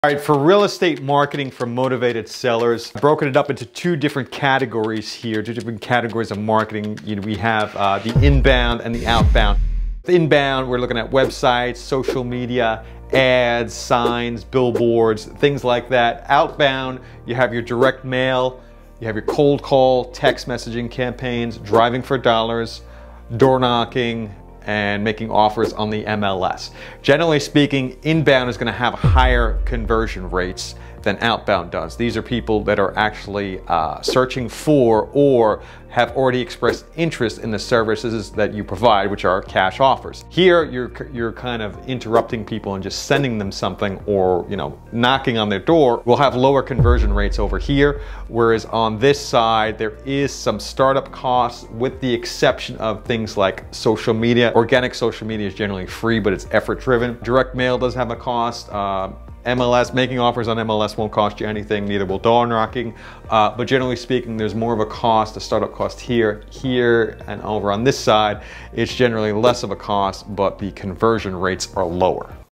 Inbound Marketing for Motivated Home Sellers
Are you a beginner real estate investor struggling to find motivated home sellers? Even if you are an experienced real estate investor I have seen many investors struggle with finding deals.  In this video, I break down the different types of marketing for finding motivated home sellers.

Remember to leave your questions in the comments section so I can answer any questions that you have.